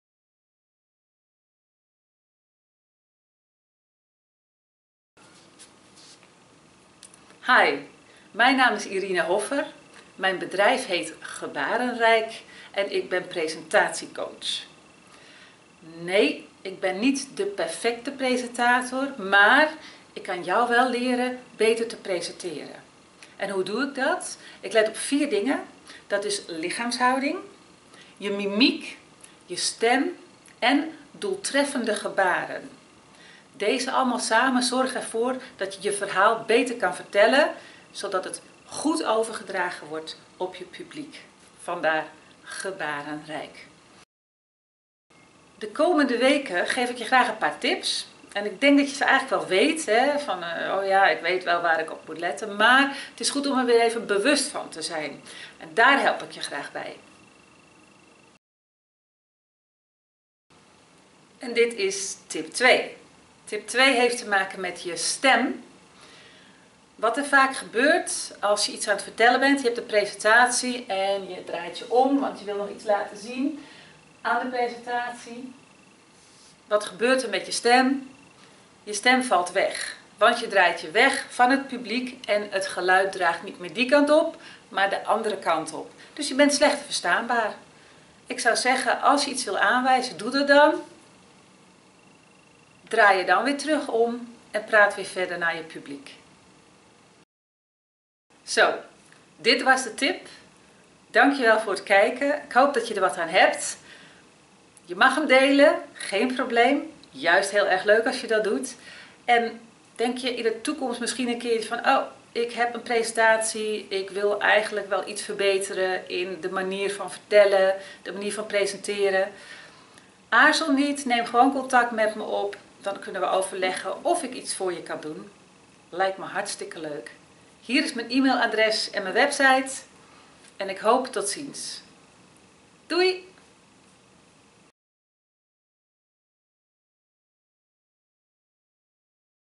presentatietip 2 : Vertel richting je publiek | GEBARENRIJK
Irina Hoffer van Gebarenrijk heeft 16 supersimpele en eenvoudige tips om aan te denken als je een presentatie geeft. Dit is presentatietip2 - Vertel richting je publiek.